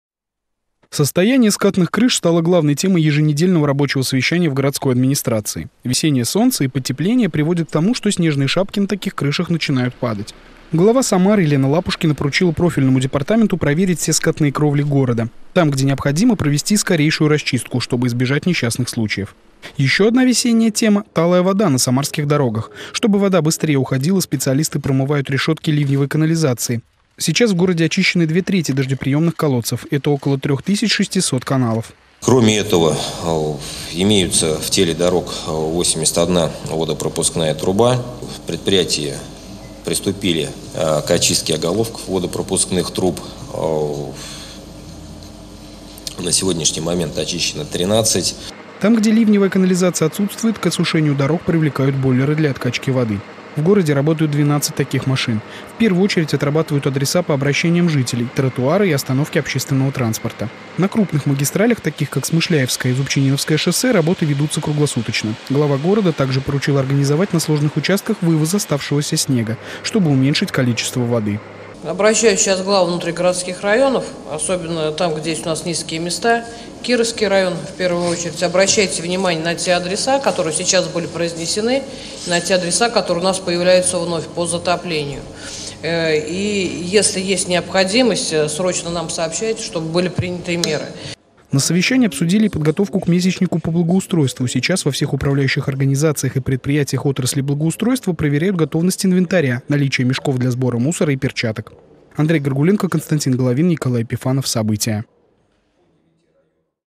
Елена Лапушкина поручила проверить все скатные крыши в Самаре
Особое внимание — снежным шапкам на крышах. Елена Лапушкина на еженедельном совещании в городской администрации поручила совместно с Государственной жилищной инспекцией и ГАТИ по благоустройству проверить все скатные кровли города и оперативно очистить их, если это необходимо. Также на рабочей встрече обсудили и другие сезонные работы по благоустройству города. Андрей Горгуленко расскажет подробнее.